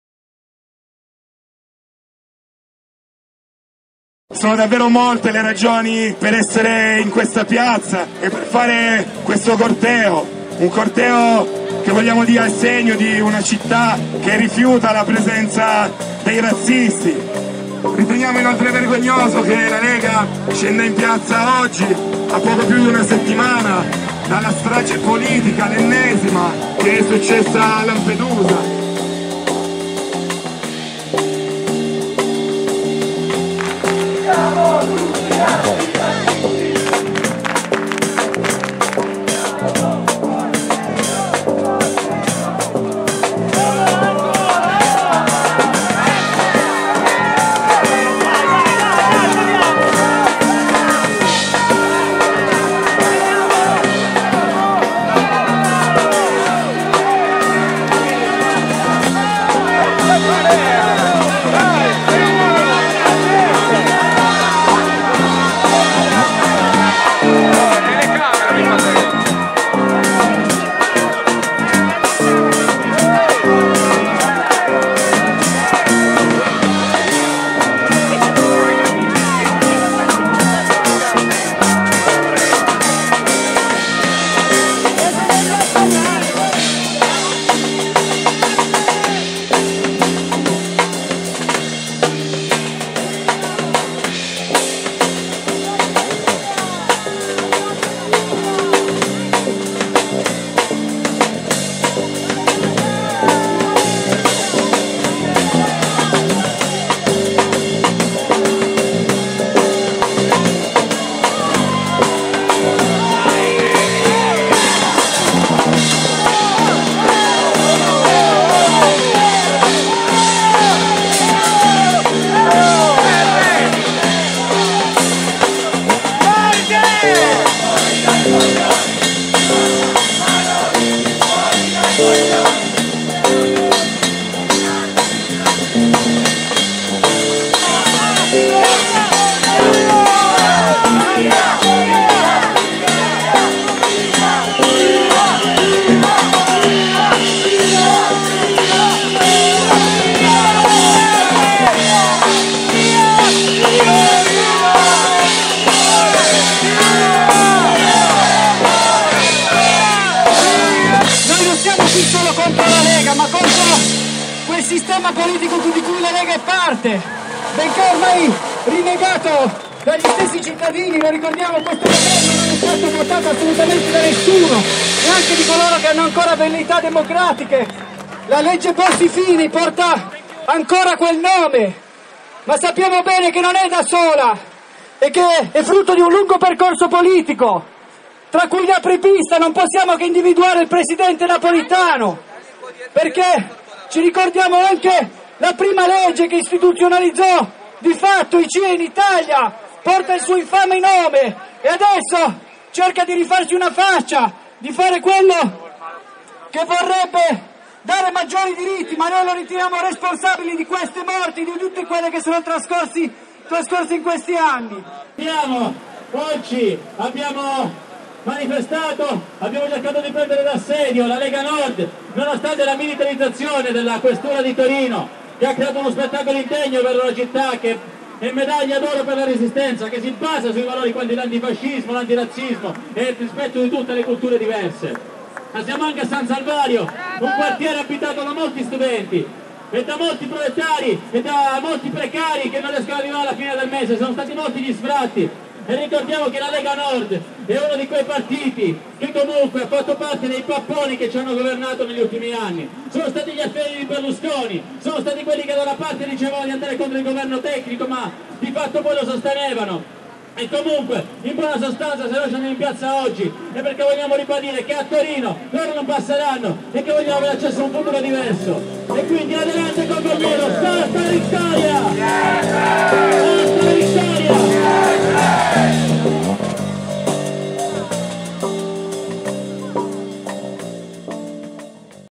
Torino #12 ottobre 2013 corteo antirazzista
La manifestazione antirazzista contro la marcia della Lega Nord annunciata da alcune settimane si è trasformata in un pomeriggio di vero e proprio assedio al blindatissimo comizio dei razzisti in camicia verde.